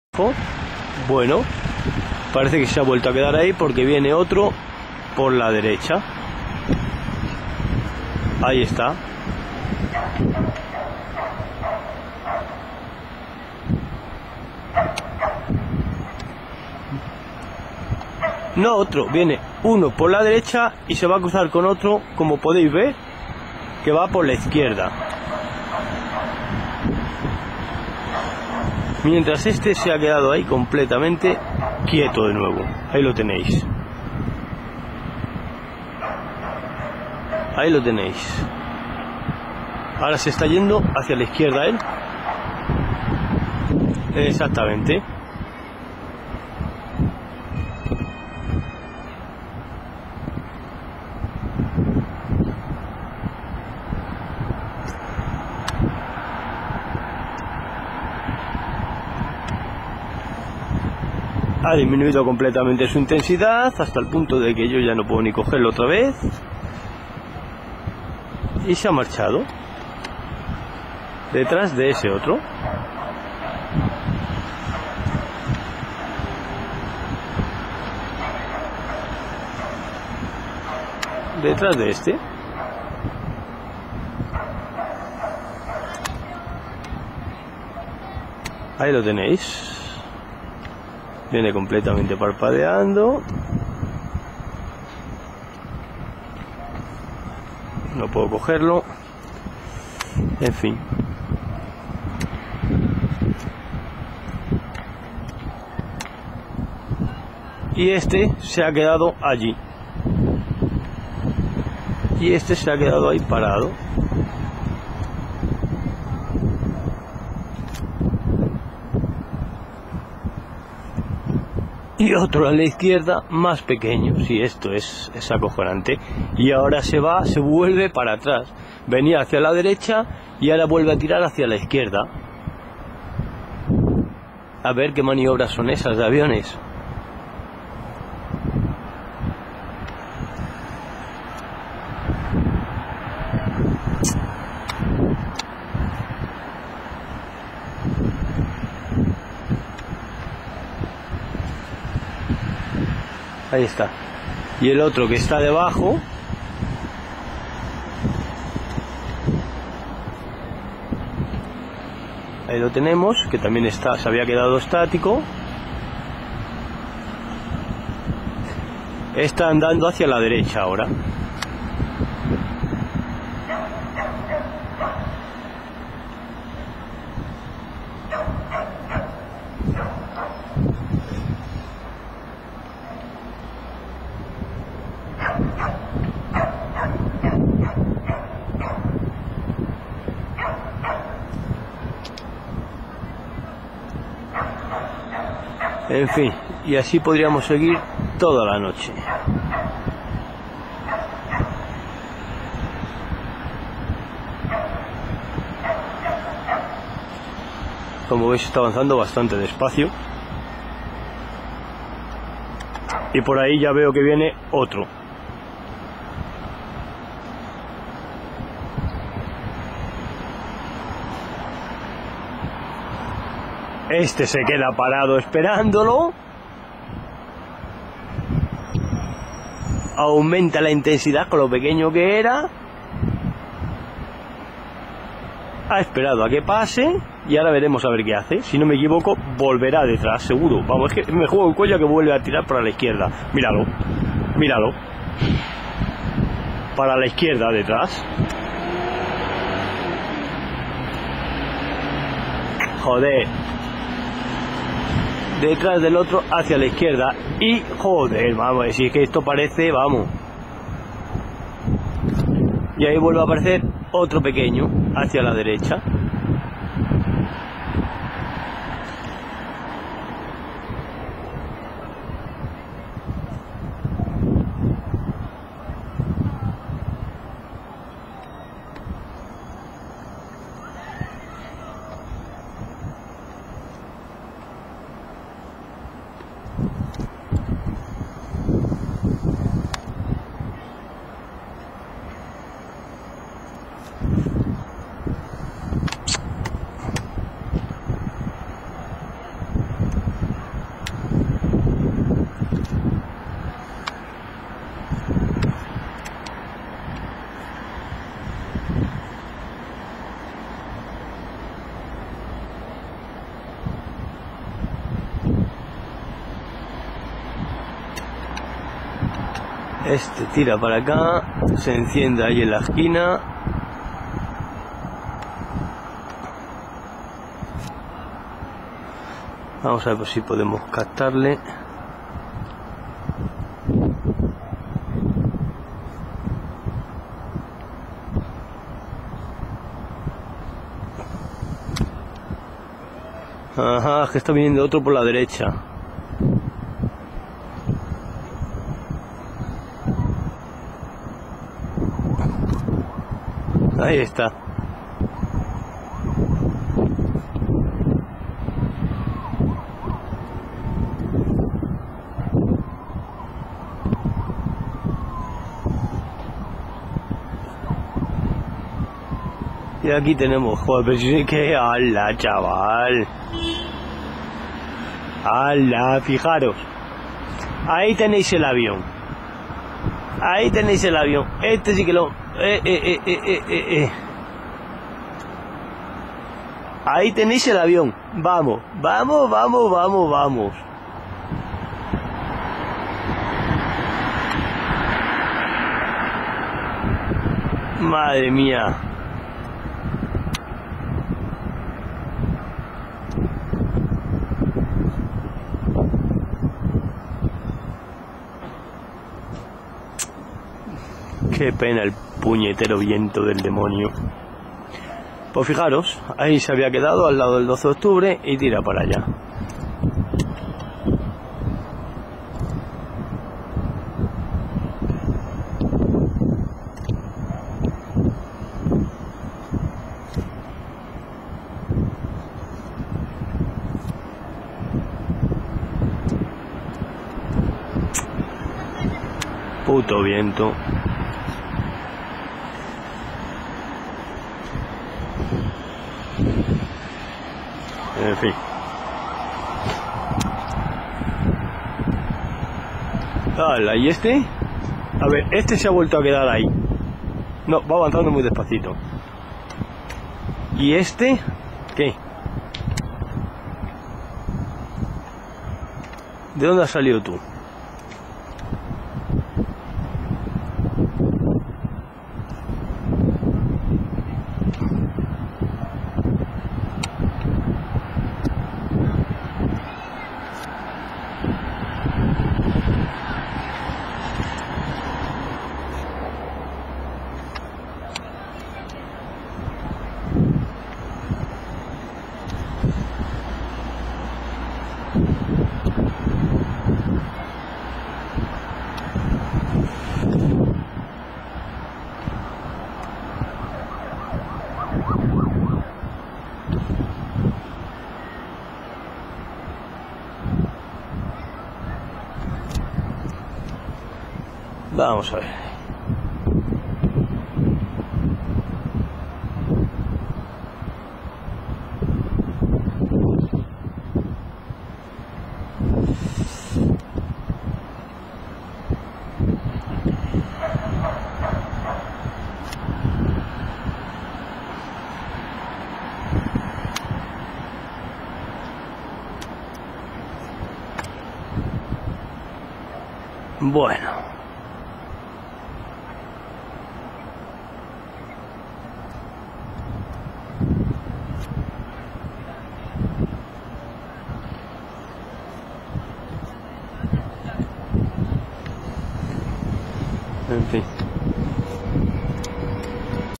OVNIS-EVIDENCIAS
04-10-11 madrid...gravado hace 2 noches. he cortado algunos minutos del principio porque era muy largo y no me cargava en jou tube.